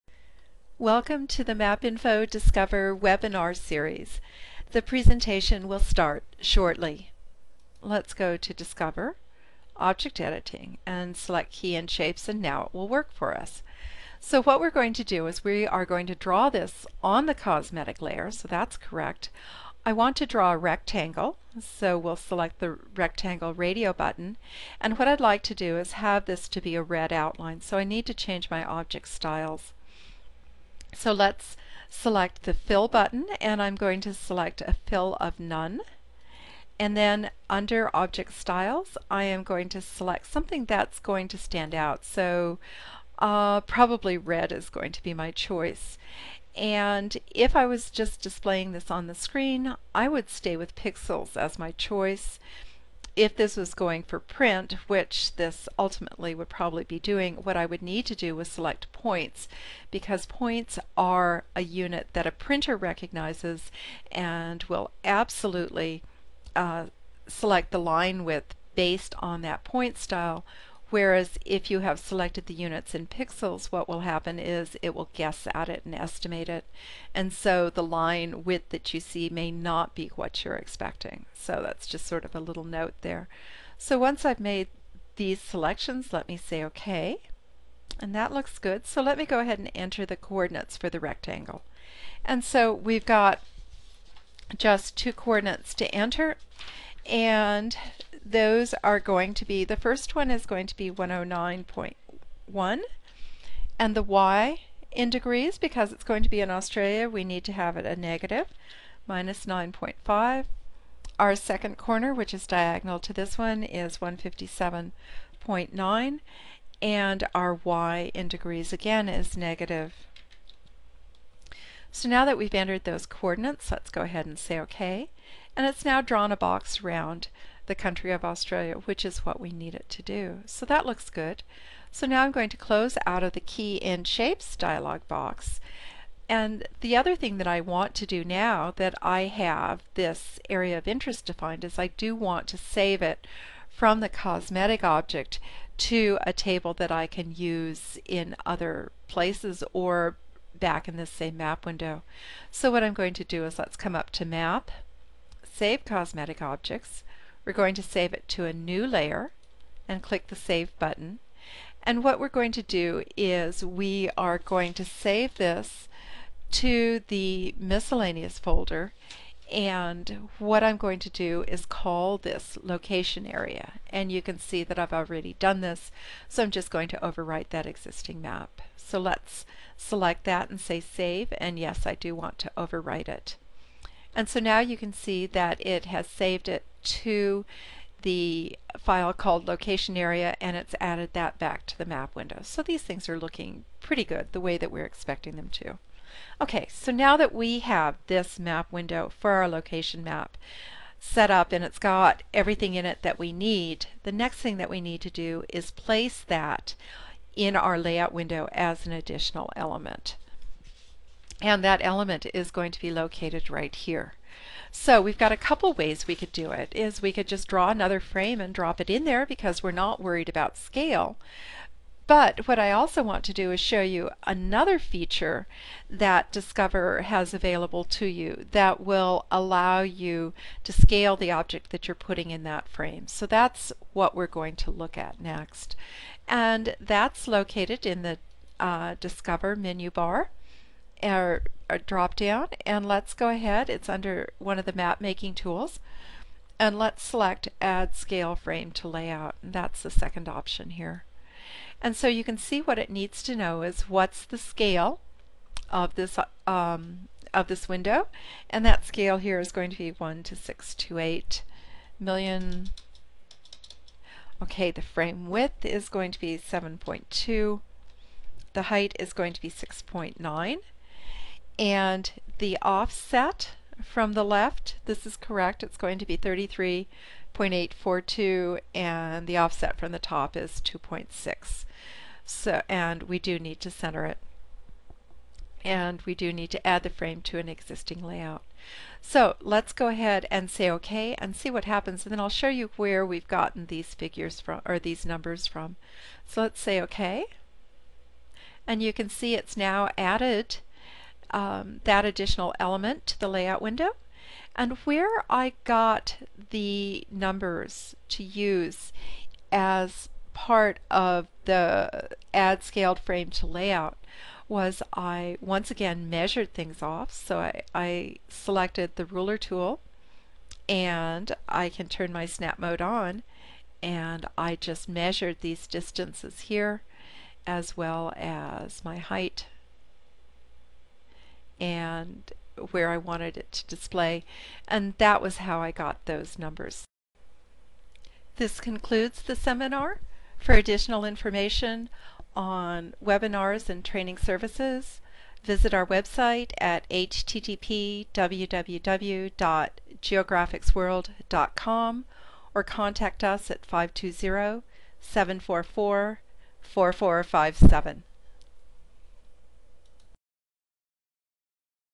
MapInfo/Discover Advanced Scaled Output clip
This is an excerpt from the MapInfo/Discover webinar series - Advanced Scaled Output.
This video builds on the previous Scaled Output webinar and covers the following topics:
-Customizing Titleblocks to include company information and logos.
-Scaled Output - Customize Layout Frame defines where on a particular size of paper, or printer the map should appear.
-Layscale Utility - enables a scalebar to be added directly to the layout window.
-Inserting Location or Index Maps using the Add Scaled Frame to Layout tool. Allows a scaled frame to be added to the layout window for the front map window.
-Layout Templates Tool - a user-defined workspace that contains a Layout window with placeholders for maps, other windows and additional elements in a specific arrangement for presentation.